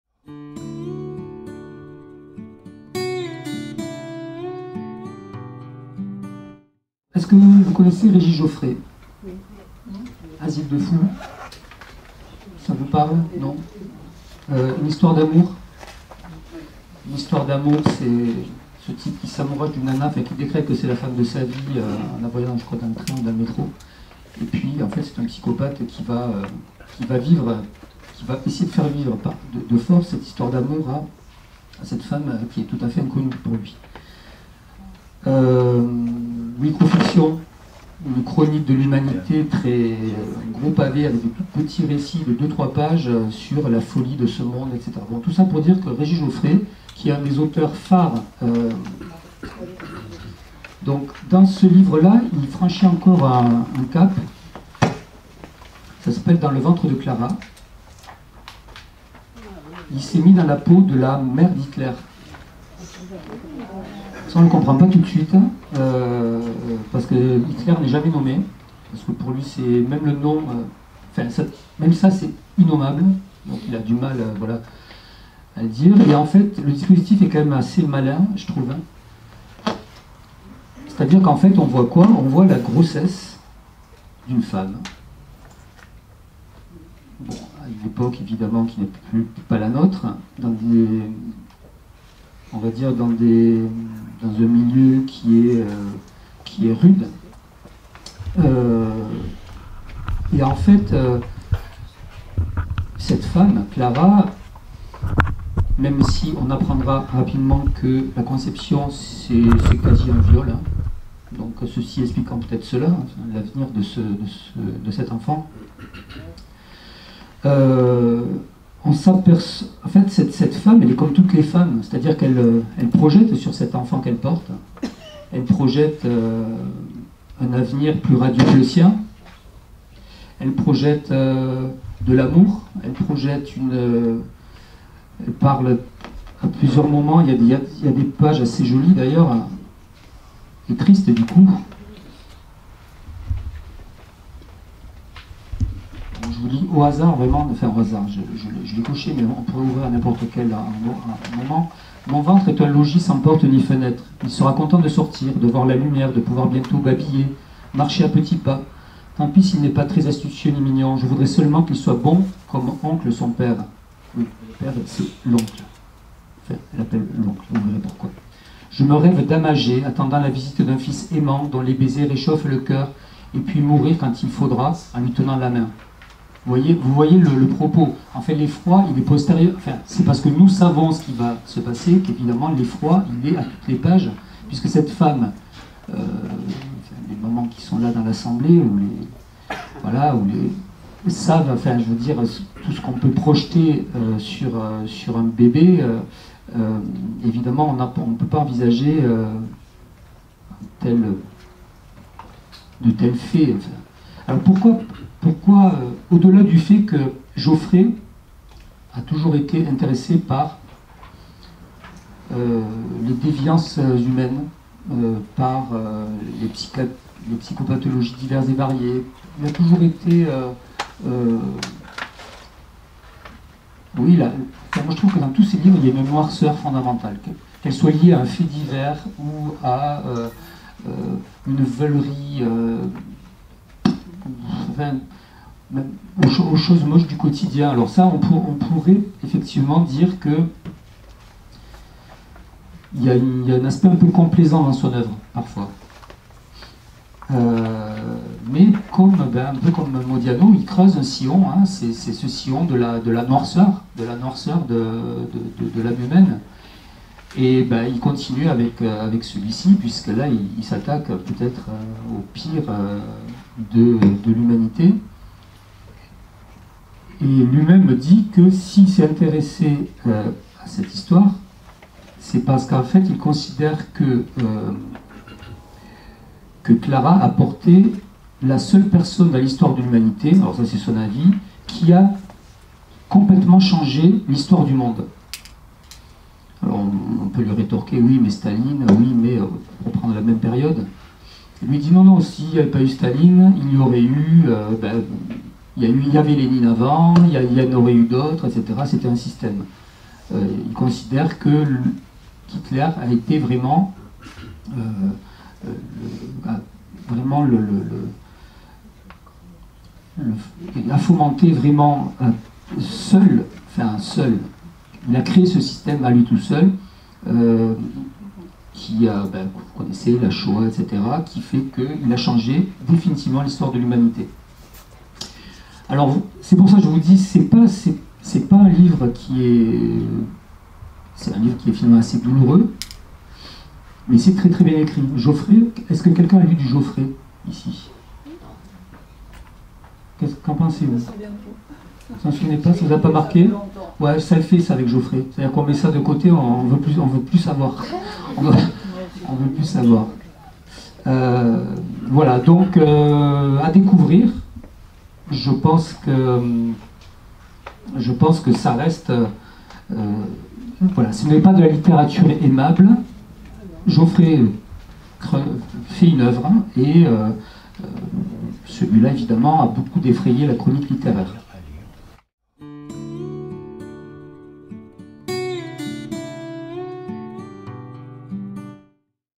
Dans le ventre de Klara de Régis Jauffret
Présentation du livre "Dans le ventre de Klara" de Régis Jauffret lors du p'tit déj littéraire de la médiathèque de l'Ancre le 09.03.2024.